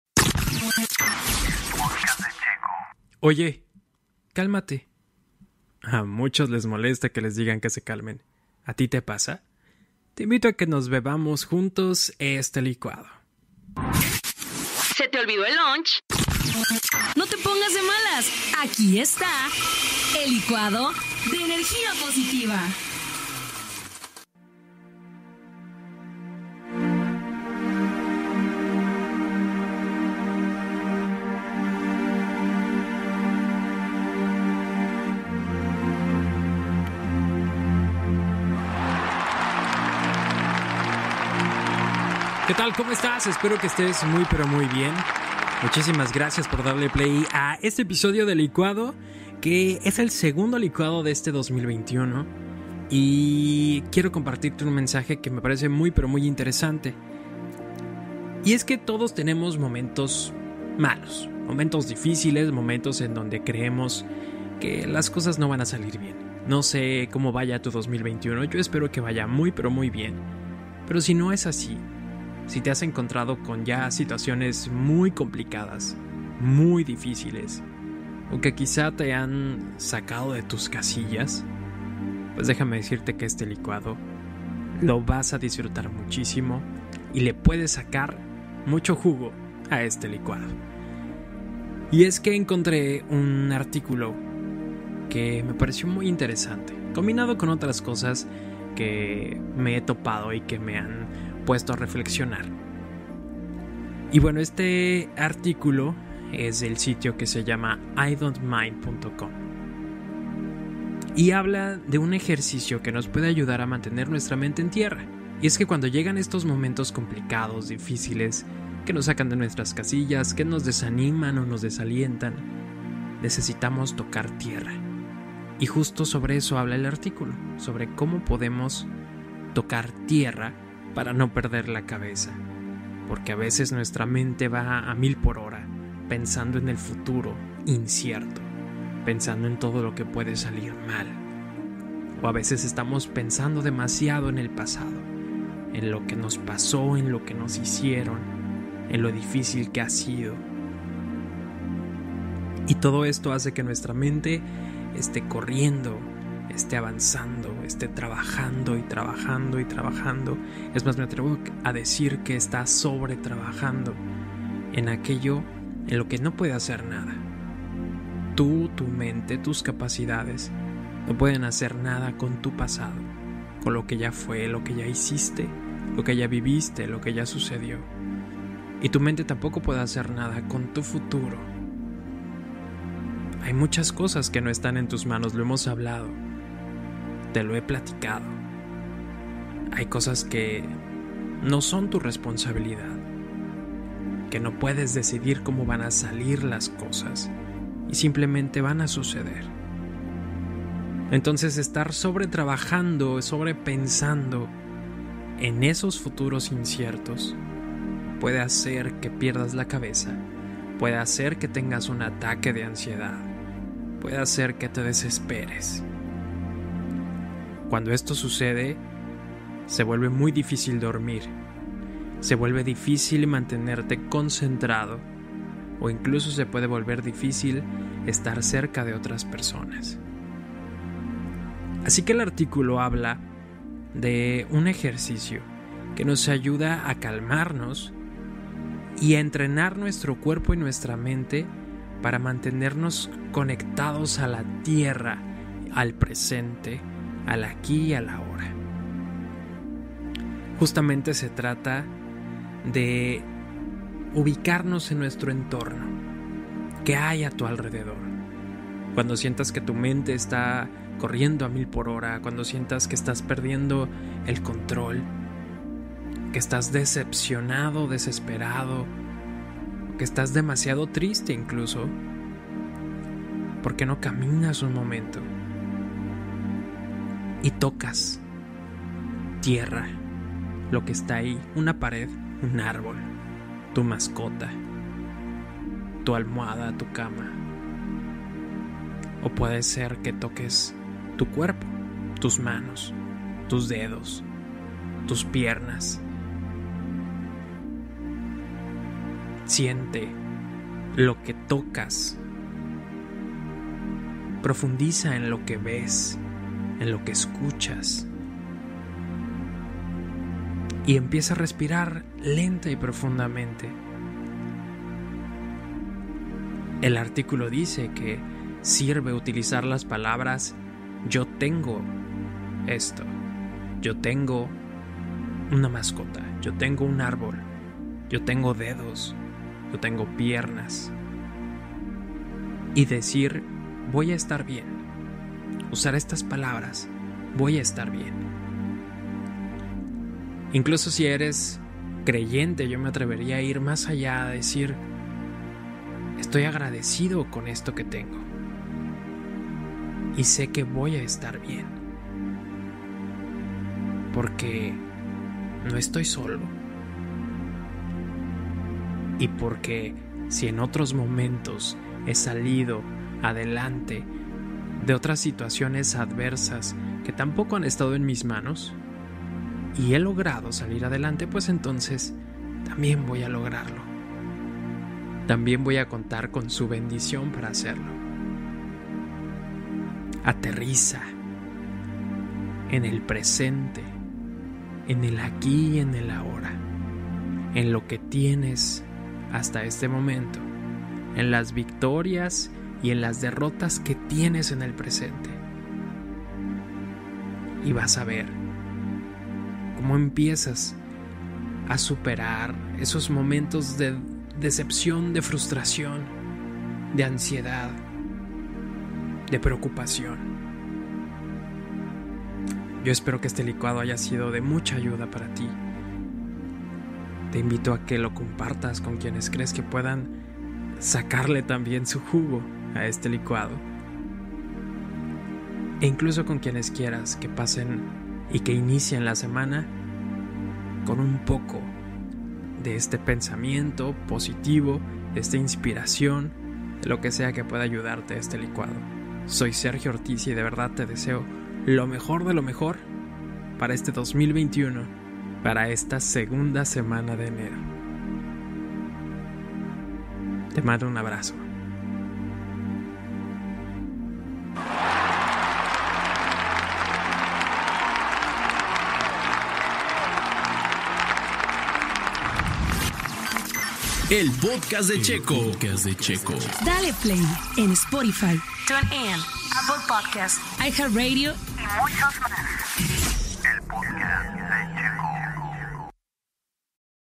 Tocar tierra - El Licuado (made with Spreaker)
Cuando vivimos momentos incómodos, decepcionantes, desafiantes o desesperanzadores podemos perder la cabeza. Nuestros pensamientos empiezan a correr a gran velocidad por el pasado y la incertidumbre del futuro.

No pierdas la cabeza, no dejes que corra, no entres en pánico o ansiedad. Toca tierra.

Te invito a beber este licuado en tu app favorita